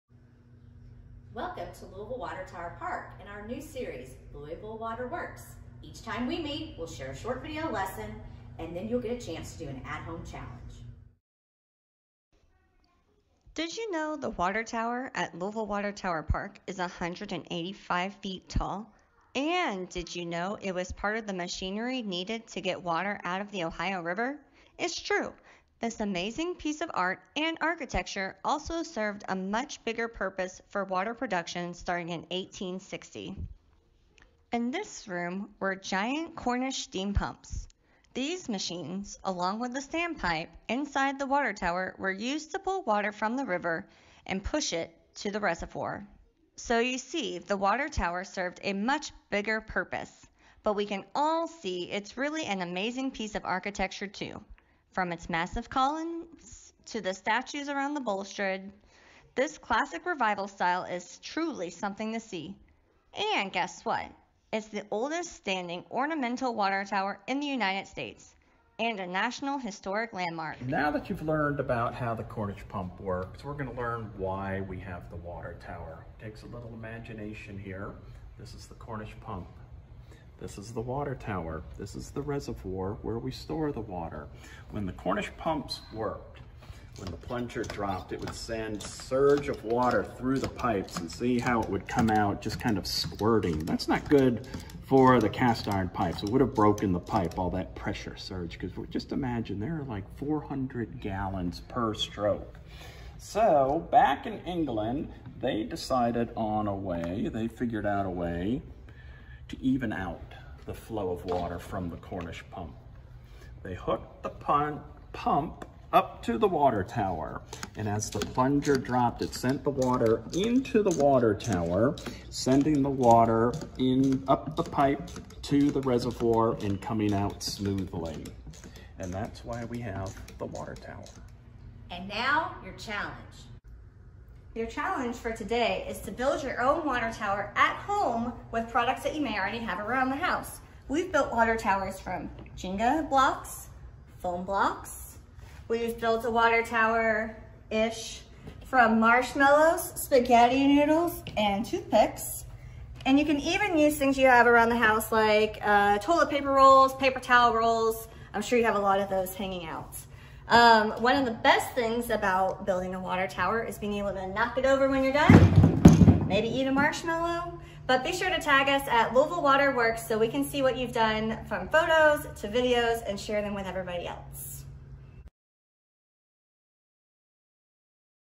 Louisville WaterWorks Museum Virtual Activities - Build a Water Tower
Explore the history of Louisville Water Tower Park with engaging lessons and hands-on activities!